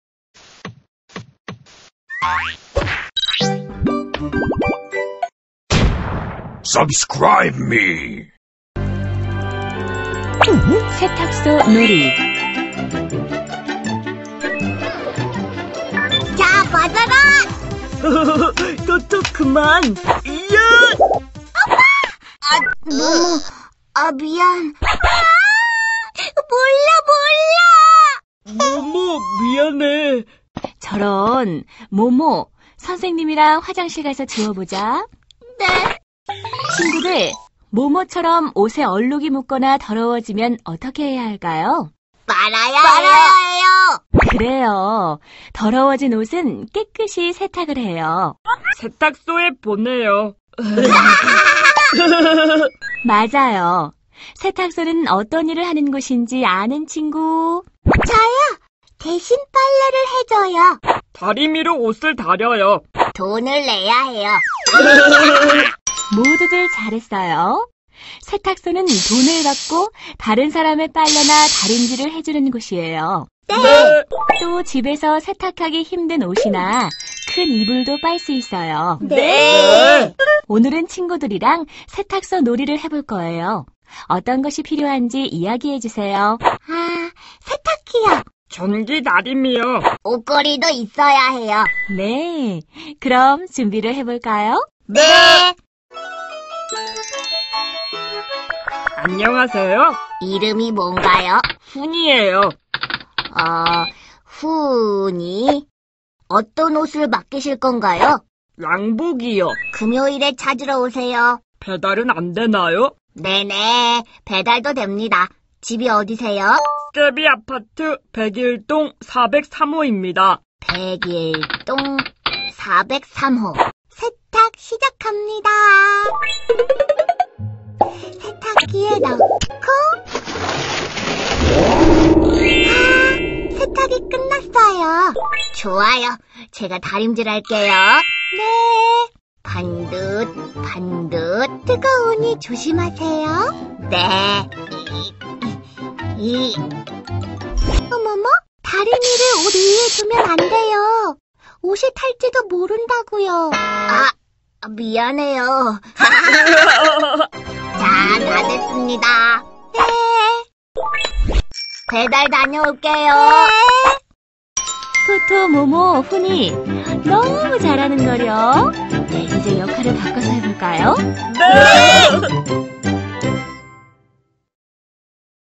세탁소 놀이 | 토토의 역할놀이 | 유아 역할극 | 깨비키즈 KEBIKIDS
세탁소에서 하는 일을 알고, 세탁, 다림질, 배달서비스 등을 역할극 해봅니다.